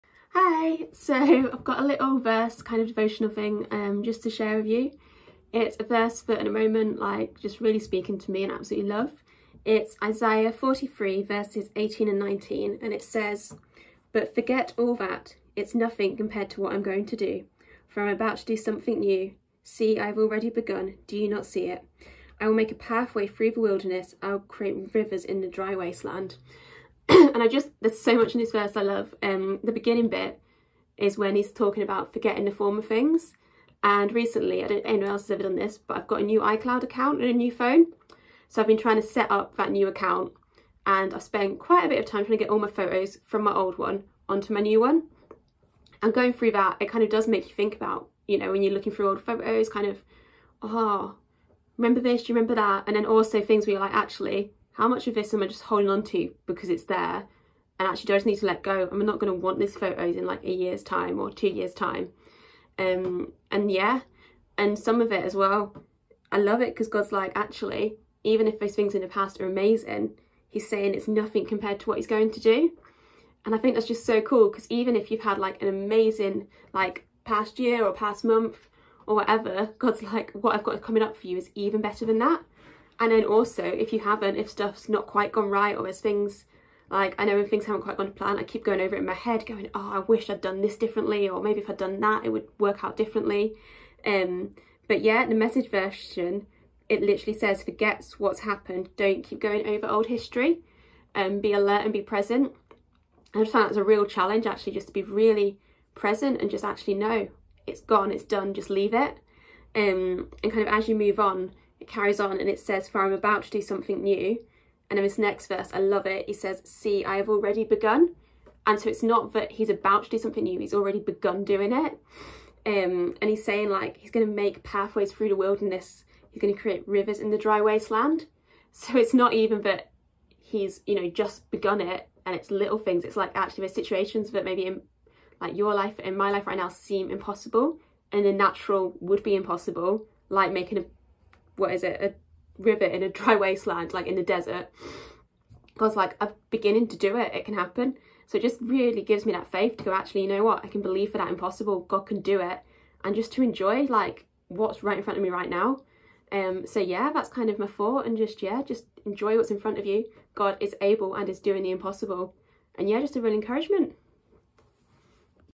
!Audacious Daily Devotionals - Thursday 17th February 2022
Today, Amy Lloyd part of our Sheffield Campus shares her video devotion with us.